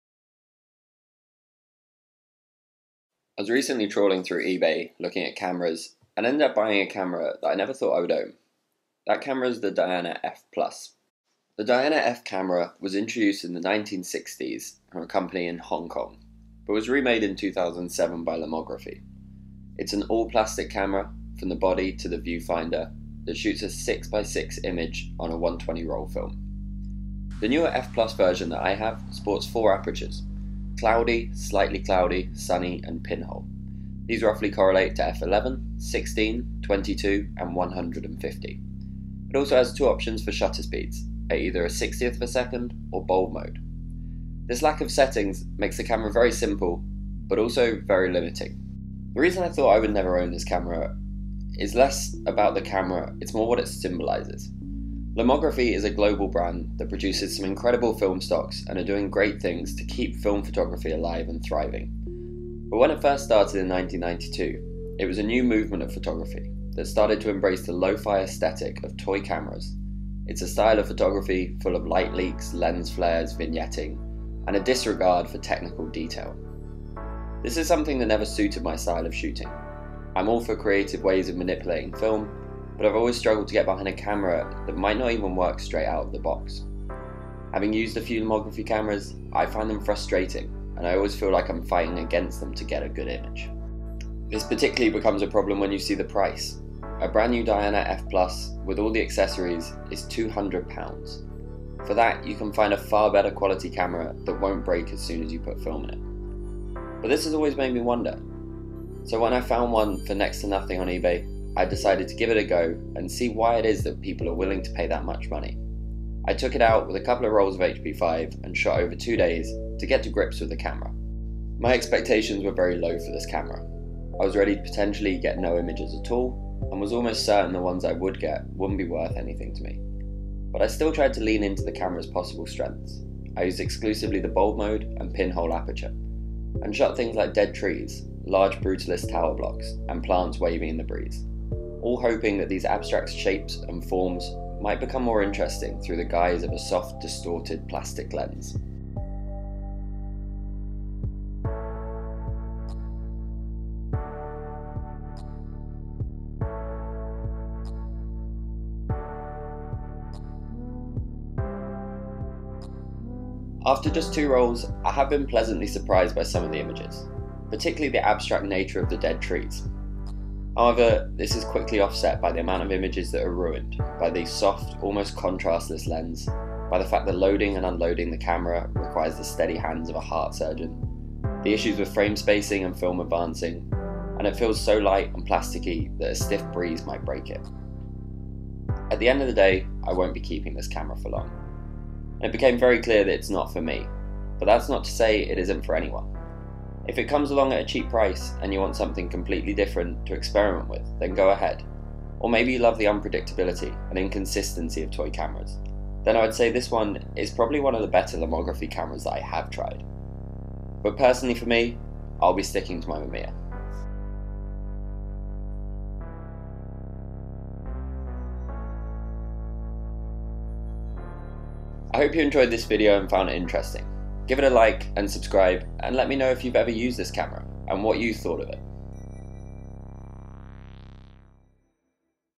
Diana F+ : Is it worth the money?
I recently got a super cheap deal on a Diana F+ so decided to give it a go and see what the hype is about.
A couple of rolls of HP5 later I had a few thoughts on the camera and Lomography in general.